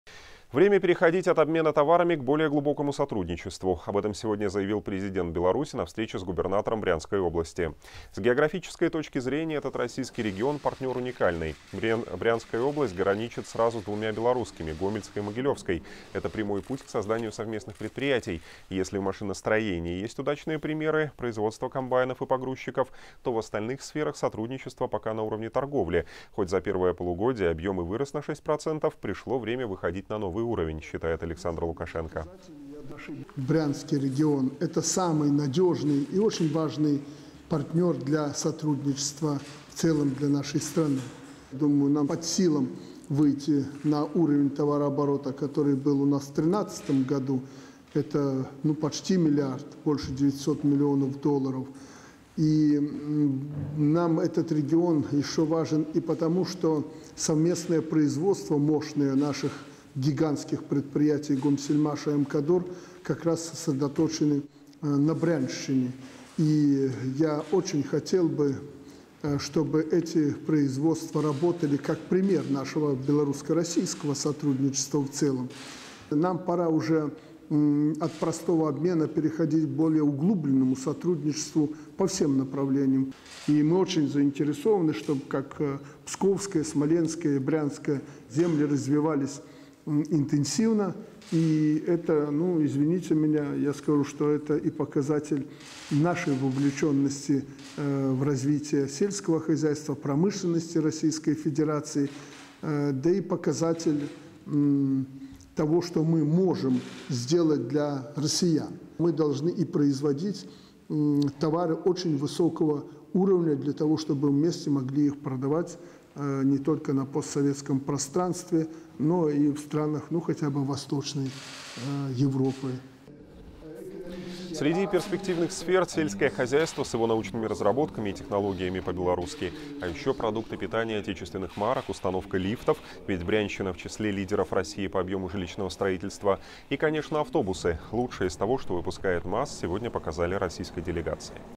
Встреча Александра Лукашенко с губернатором Брянской области Александром Богомазом
С географической точки зрения этот российский регион – партнёр уникальный. Брянская область граничит сразу с двумя белорусскими – Гомельской и Могилёвской. Это прямой путь к созданию совместных предприятий.